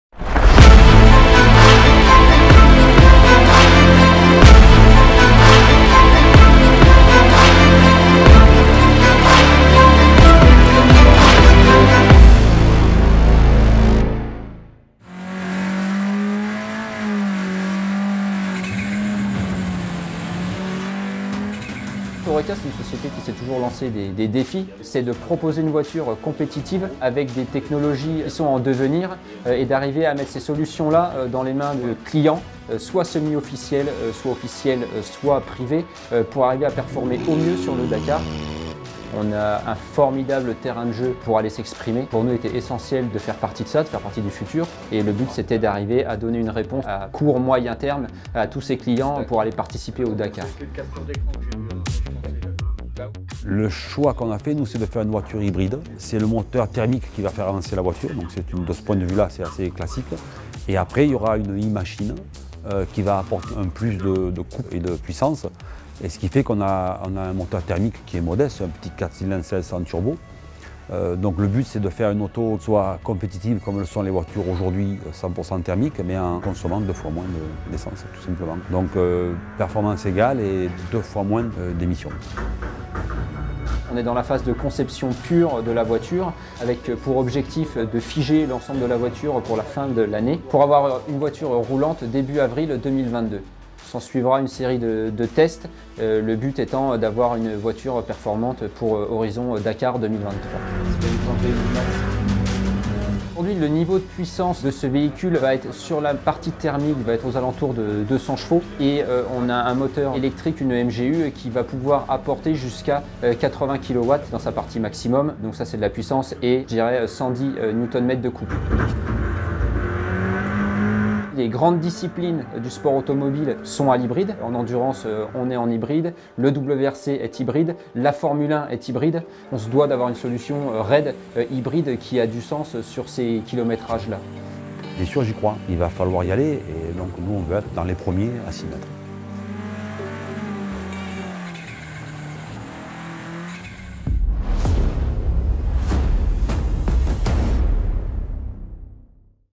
DAKAR FUTURE - A hybrid car for Oreca
In 2021, ORECA is taking on a new challenge: the design and development of a hybrid car to participate in "Dakar Future", the Dakar energy transition program.
Alongside Philippe Gache and his SMG team, the objective of the ORECA Magny-Cours teams is simple: to create a competitive hybrid car that will consume half as much as a thermal vehicle like a T1. See you in 2023 to see this project come to life in the dunes!